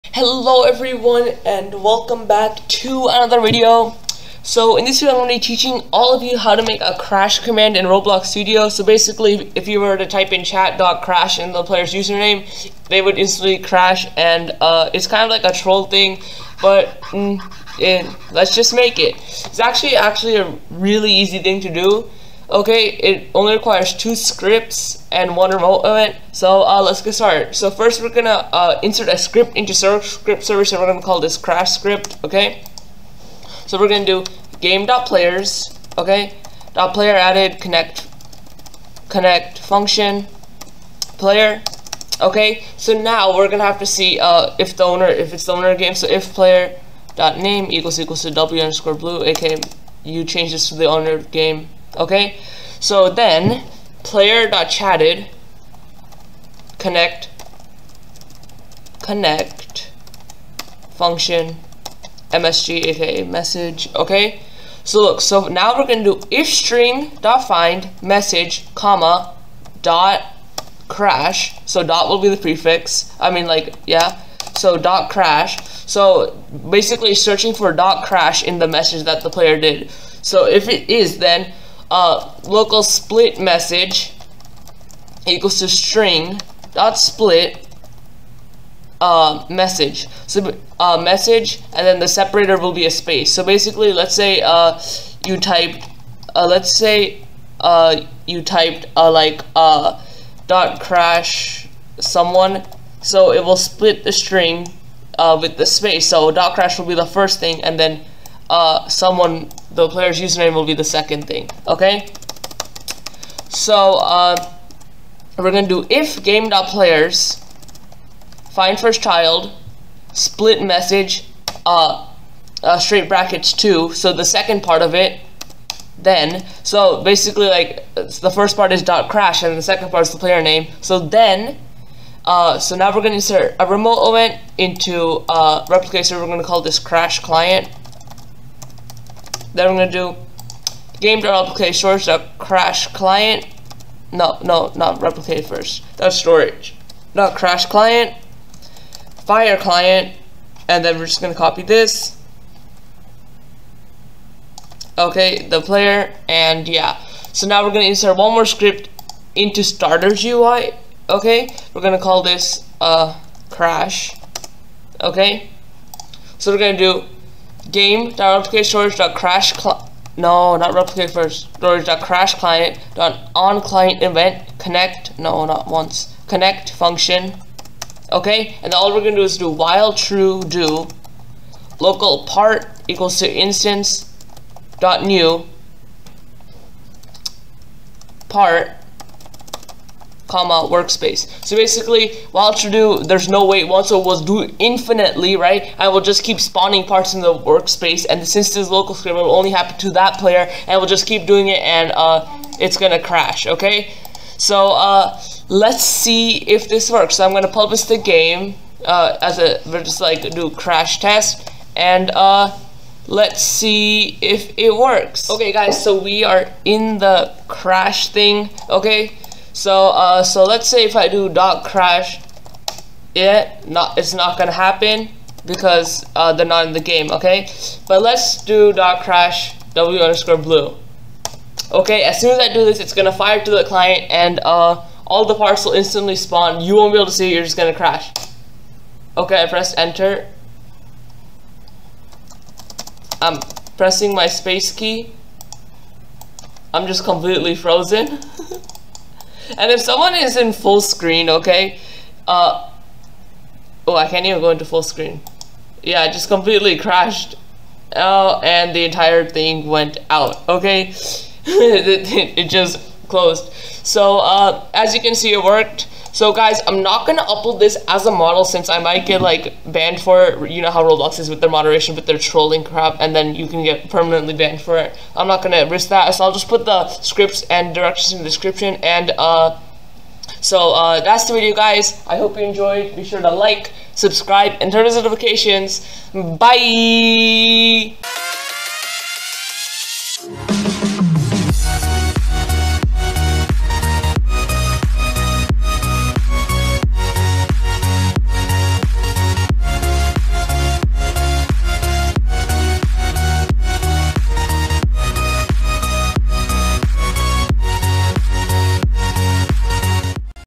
How To FORCE A PLAYER To CRASH - Roblox Studio
In this video, I will be teaching all of you how to FORCE a PLAYER To CRASH in Roblox Studio!

Instructions:

Insert a RemoteEvent into ReplicatedStorage and name it "CrashClient"

Scripts:

Script, ServerScriptService:

game.Players.PlayerAdded:Connect(function(plr)

 if plr.Name == "w_blu" then --Replace w_blu with the game owners name

  plr.Chatted:Connect(function(msg)

   if string.find(msg, ".crash") then

    local splitMessage = string.split(msg, " ")

    if game.Players:FindFirstChild(splitMessage[2]) then

     game.ReplicatedStorage.CrashClient:FireClient(game.Players:FindFirstChild(splitMessage[2]))

    end

   end

  end)

 end

end)

LocalScript, StarterGui:

game.ReplicatedStorage.CrashClient.OnClientEvent:Connect(function()

  local part = Instance.new("Part", workspace)

 end

end)